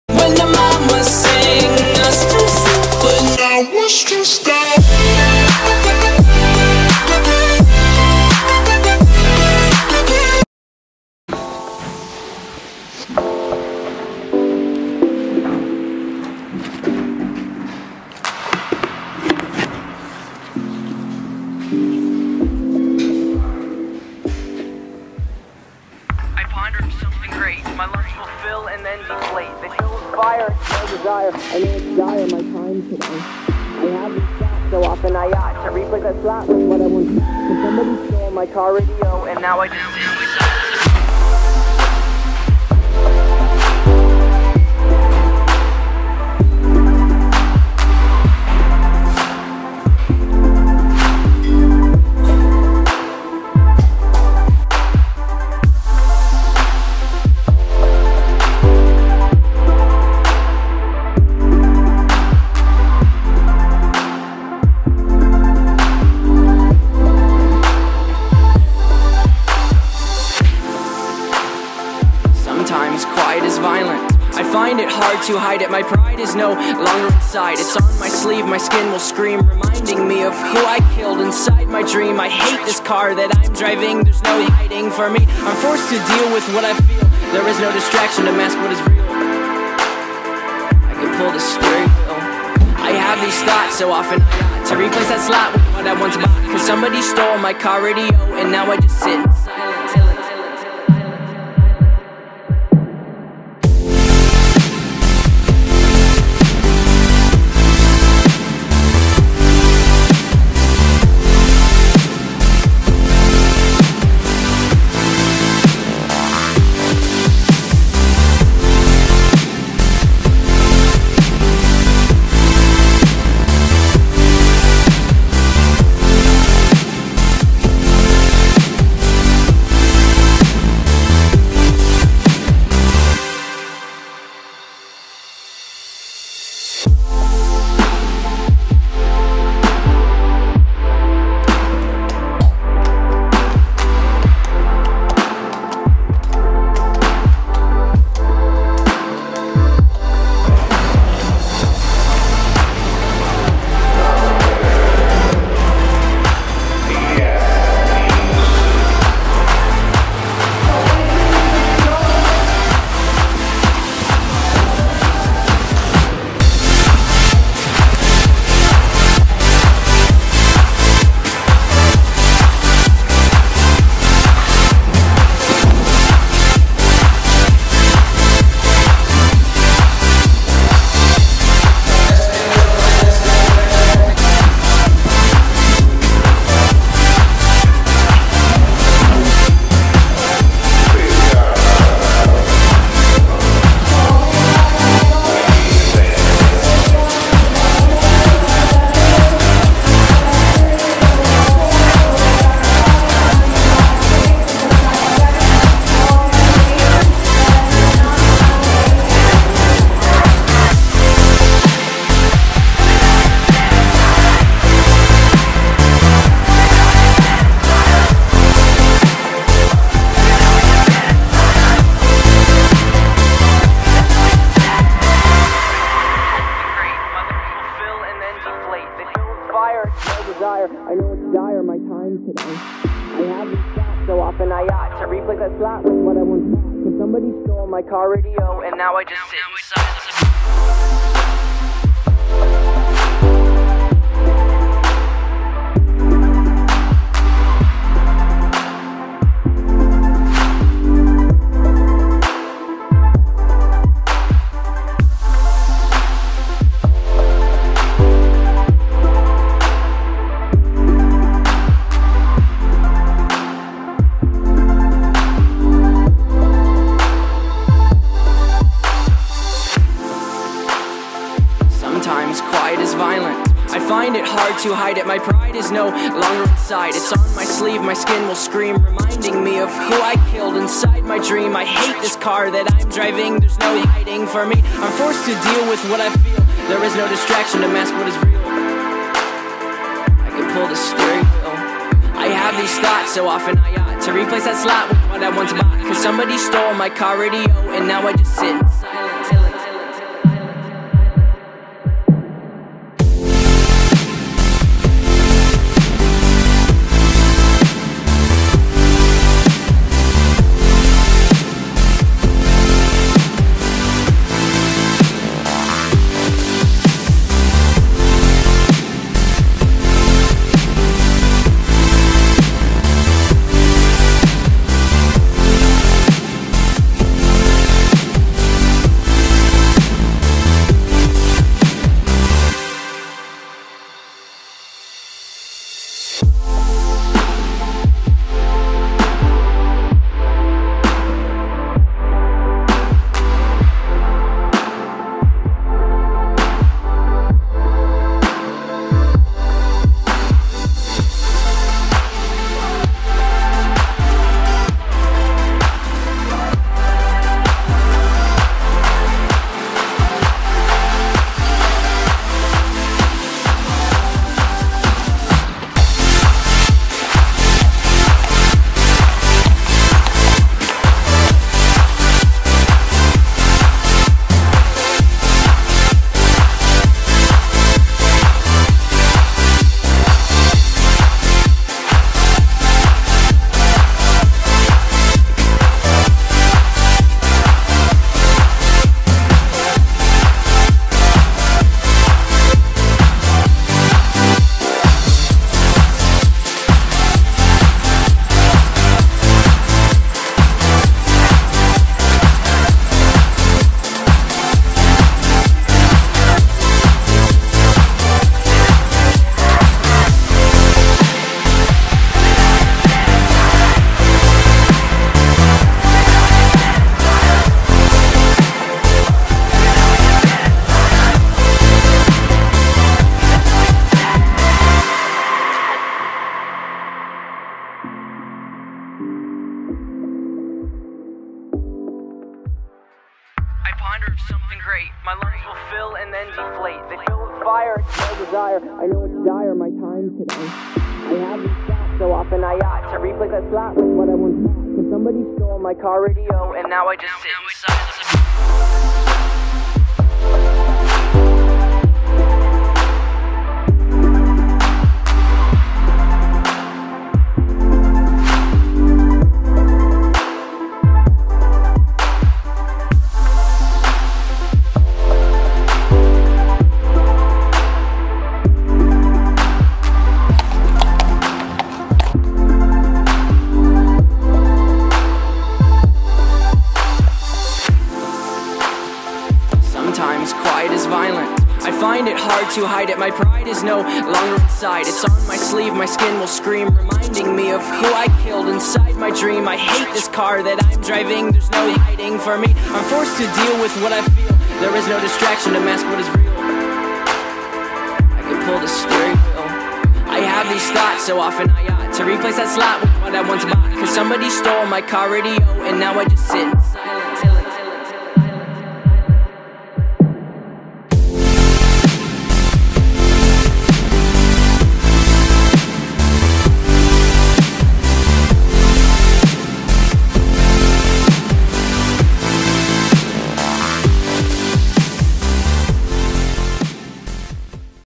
CAVAN CHAMPIONSHIPS DAY 2 | Horse Show Vlog
Cavan Spring Horse Championships with Khandi

Saturday Results:
1.10 - 4 faults
1.20 - 4 faults (Khandi's first 1.20)

Horses in this video:
Khandi (Kovali K) : 6yr old 16.2hh bay gelding by Candillo Z out of a Guidam mare